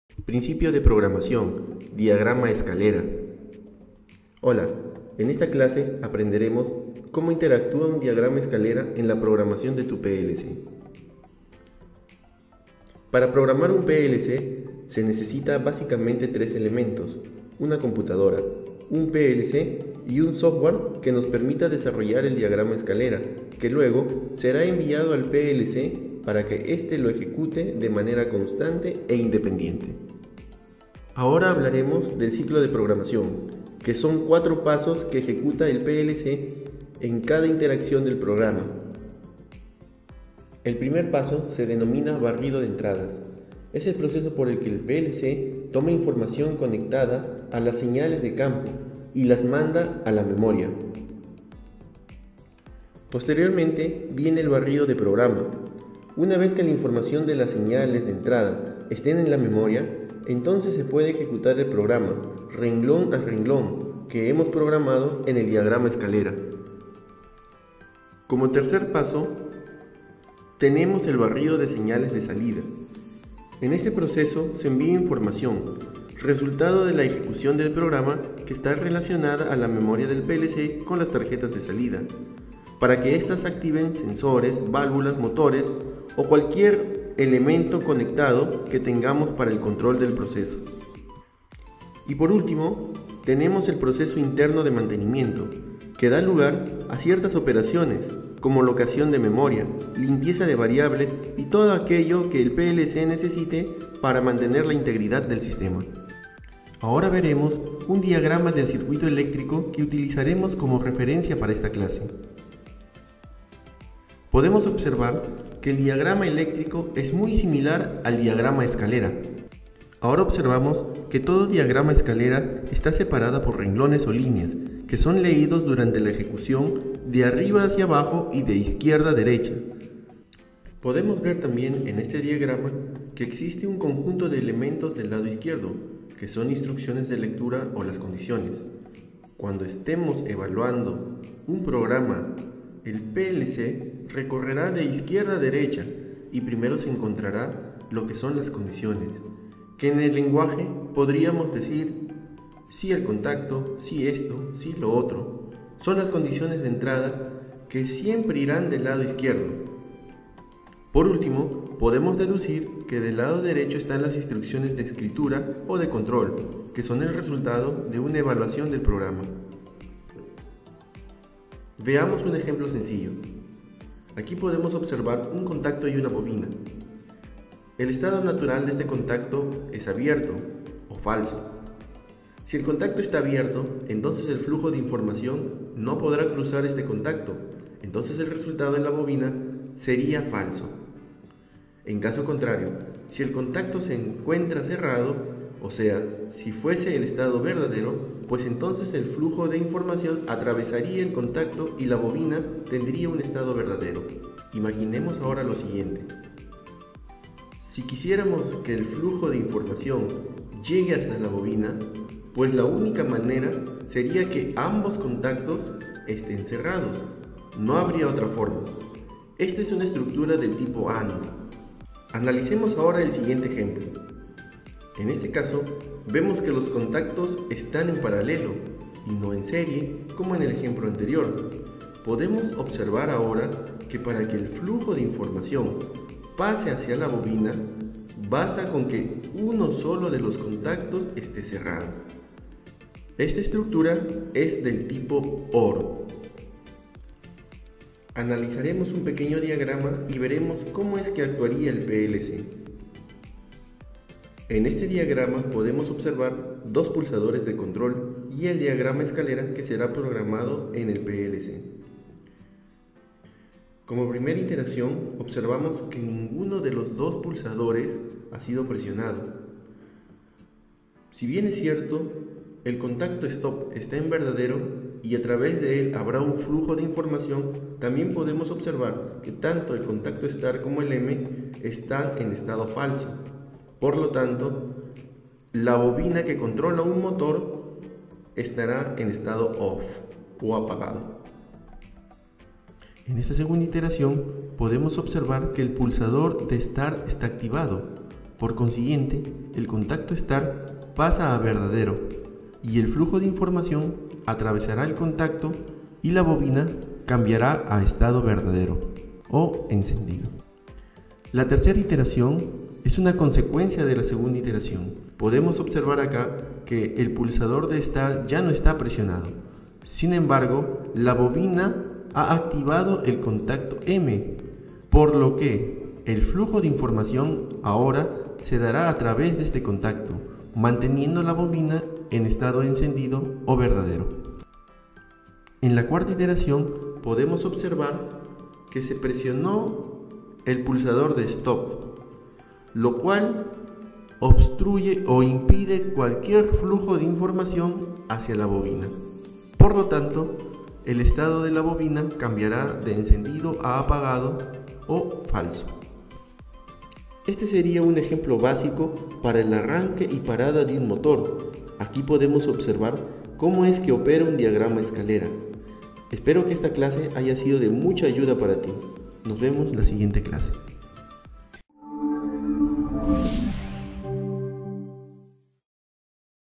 Principios de Programacion de PLCs: Diagrama Escalera
En este video te quiero compartir información acerca del lenguaje Ladder o Escalera, para que entiendas mejor como funciona.

Saludos